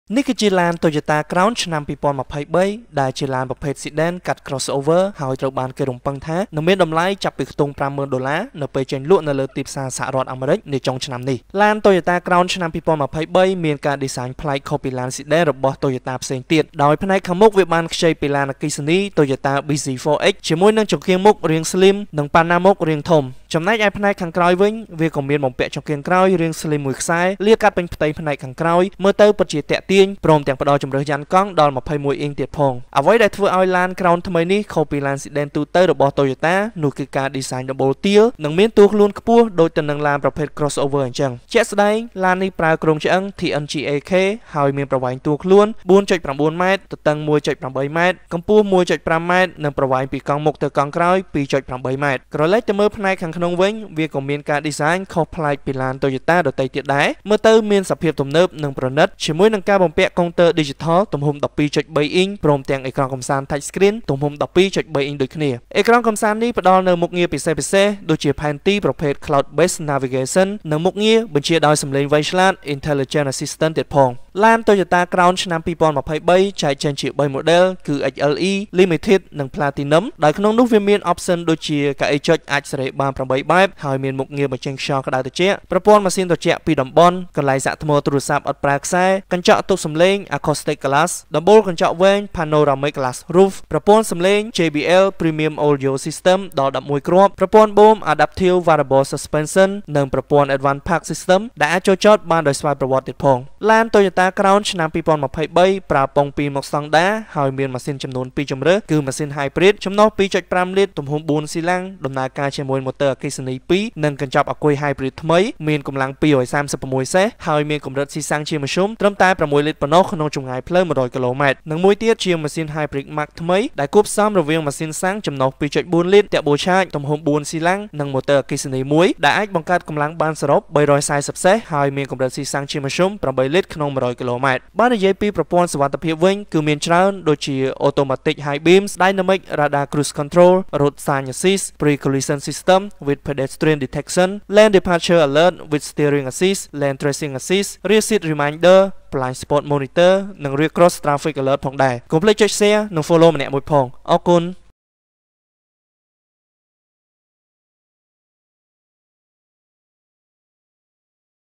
ឡាន Toyota ថ្មី ជិះ១០០គ.ម ស៊ីតែ៦លីត្រ😘 | Toyota Crown 2023
ឡាន Toyota ថ្មី ជិះ១០០គ.ម ស៊ីតែ៦លីត្រ😘